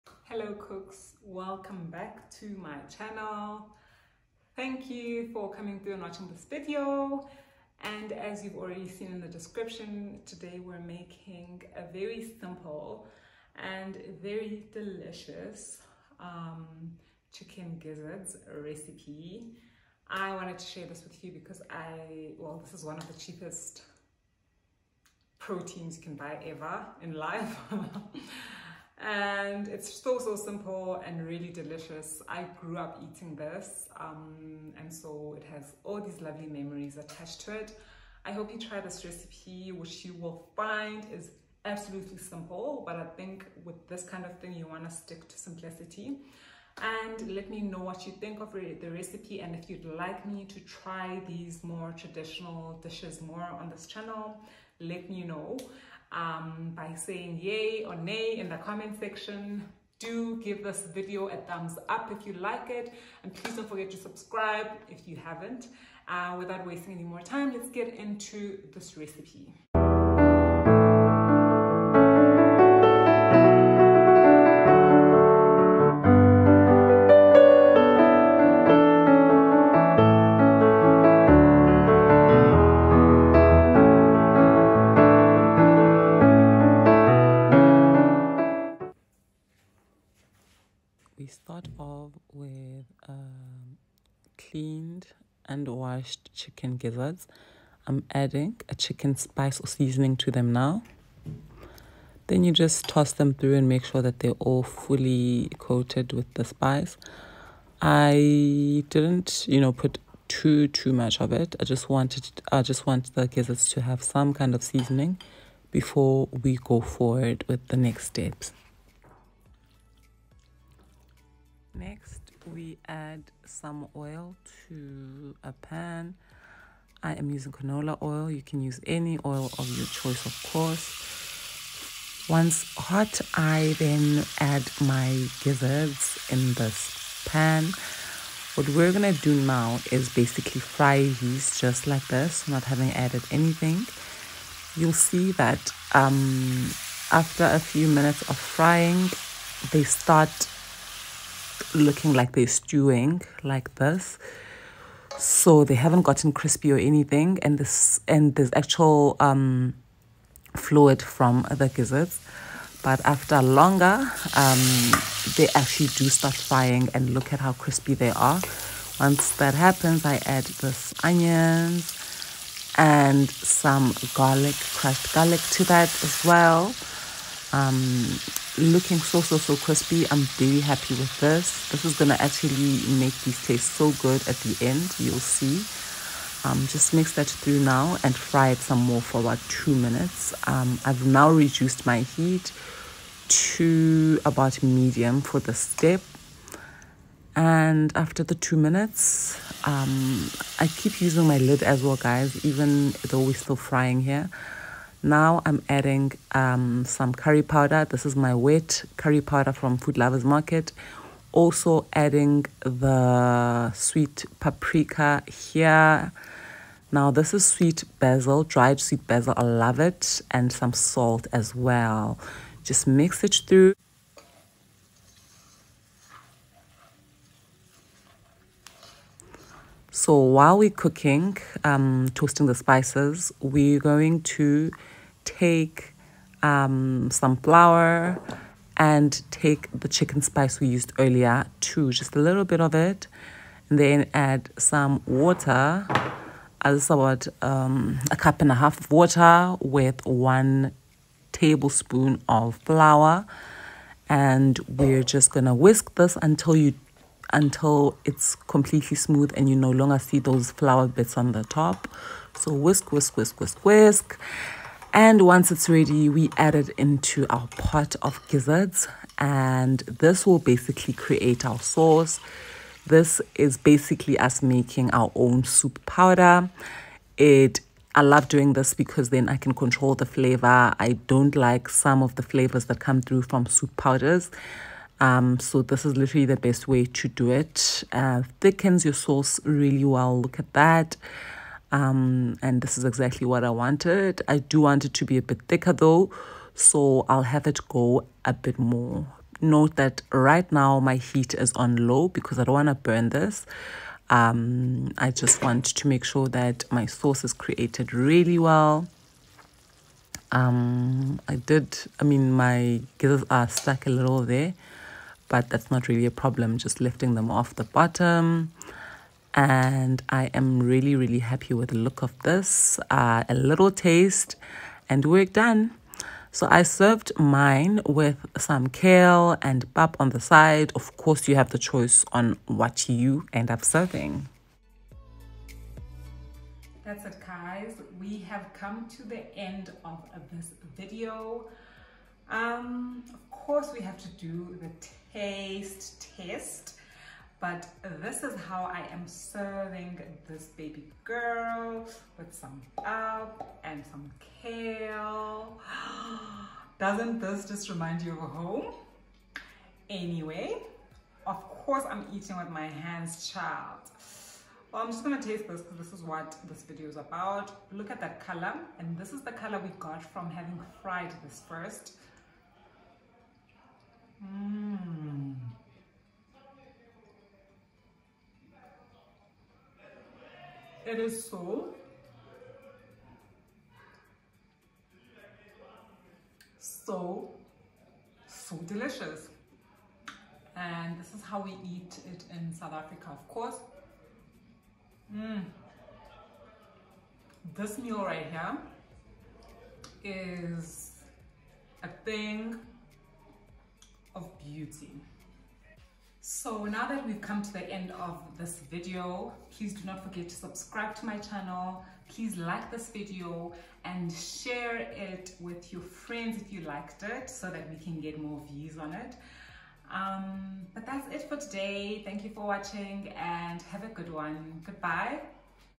Chicken gizzards recipe | how to cook chicken gizzards | SAYouTuber
Ingredients:

500g clean chicken gizzards
3 tsp chicken seasoning
2 tbsp canola oil
1 onion
1 tsp crushed garlic
1 tsp curry powder
1 tsp sweet paprika
1 tsp dried sweet basil
1/4 tsp salt
1 tbsp flour
1.5 cups water